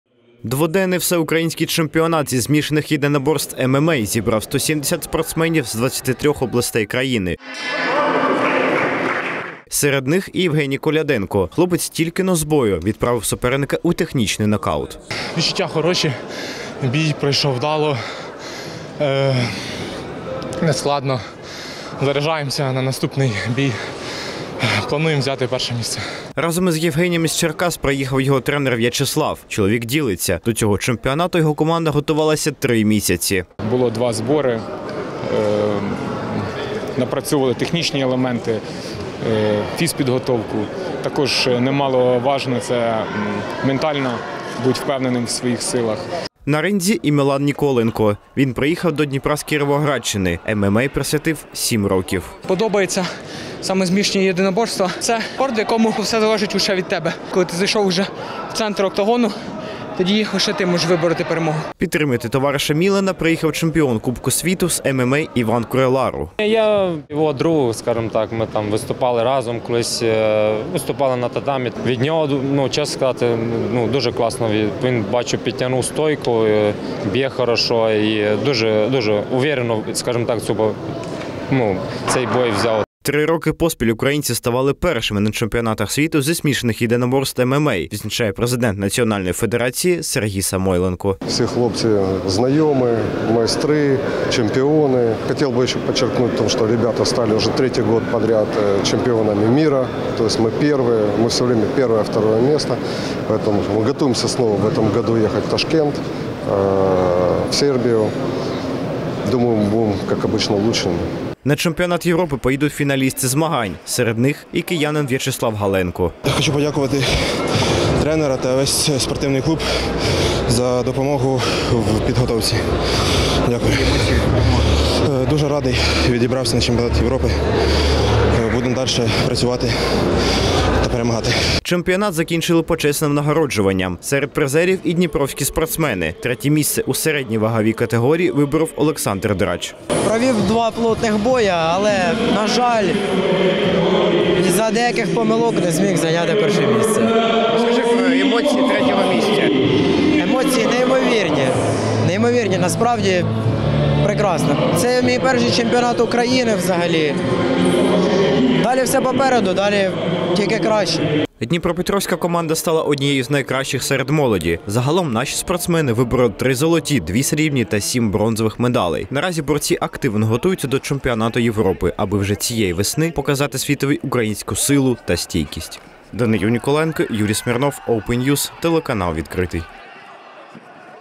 У Дніпрі пройшов Всеукраїнський чемпіонат зі змішаних єдиноборств ММА
Показували спортивний професіоналізм і справжню силу. У Дніпрі пройшов Всеукраїнський чемпіонат зі змішаних єдиноборств ММА. На нього з’їхалися 170 борців із 23 областей України. Про те, як пройшли змагання і хто представлятиме нашу державу на Чемпіонаті Європи - дивіться далі у сюжеті.